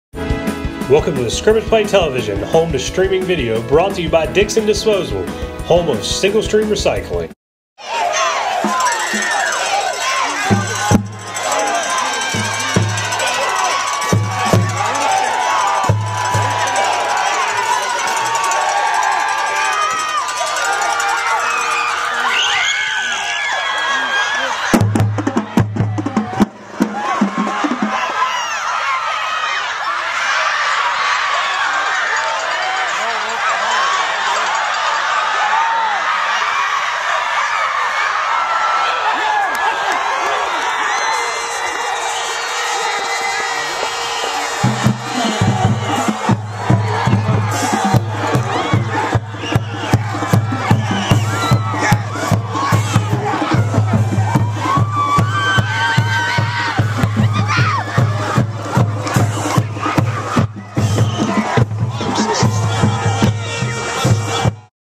First look: final play and celebration in Louisa's 32-29 victory over Western
Louisa spoils Western Albemarle's perfect regular season with an incredible effort. Here's the final play by Western that includes several laterals and the ensuing Louisa celebration.